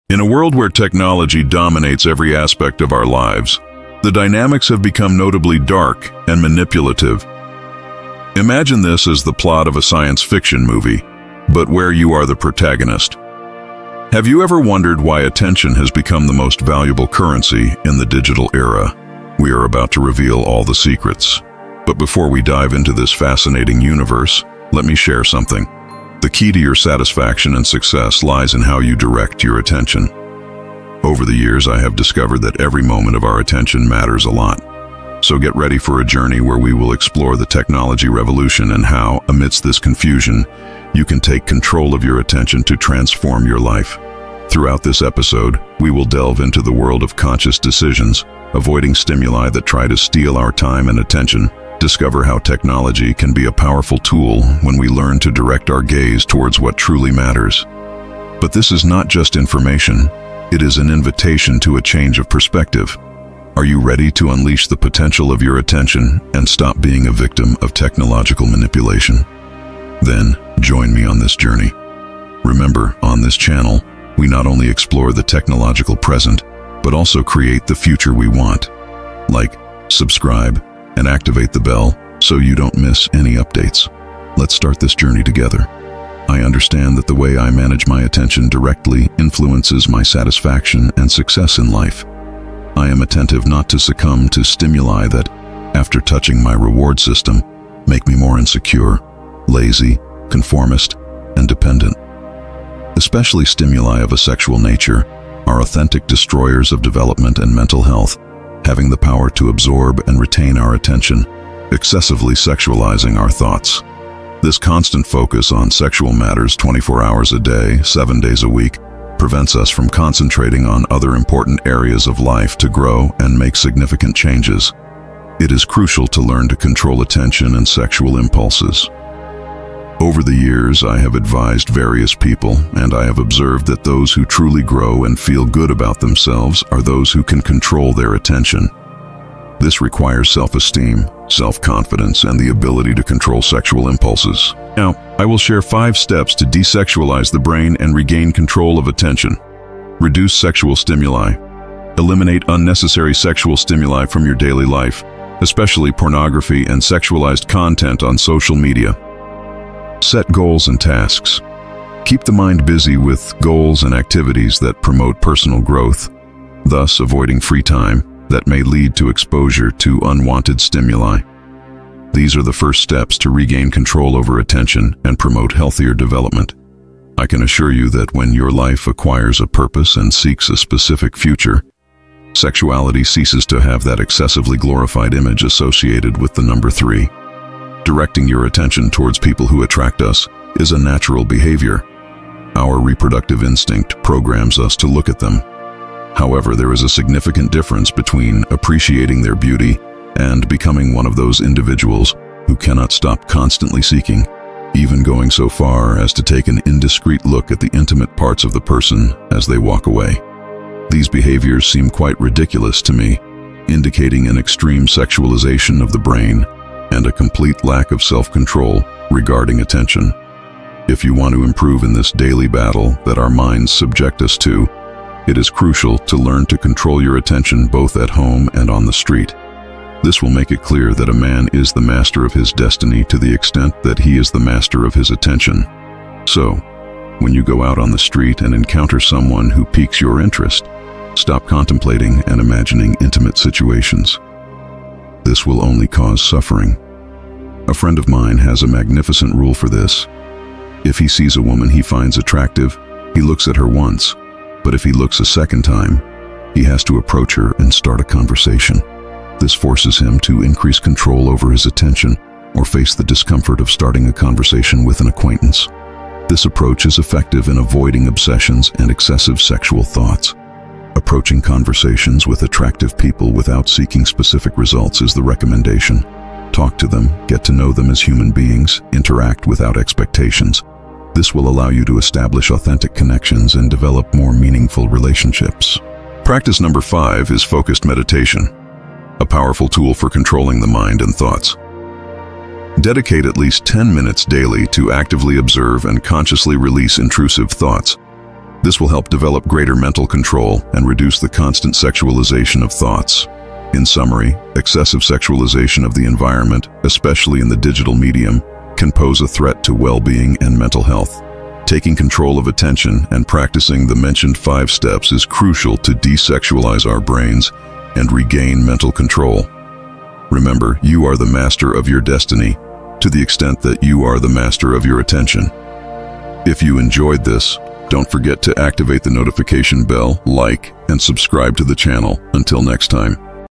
DESEXUALIZE your BRIAN and master your ATTENTION - STOICISM
In this fascinating video, we explore how Stoic philosophy can transform the way you perceive and direct your attention. Discover powerful strategies to desexualize your mind, freeing it from unnecessary distractions and enabling you to achieve a greater focus on your goals. Immerse yourself in the ancient Stoic teachings that provide a unique perspective on controlling your thoughts and emotions to cultivate a fuller and more centered life.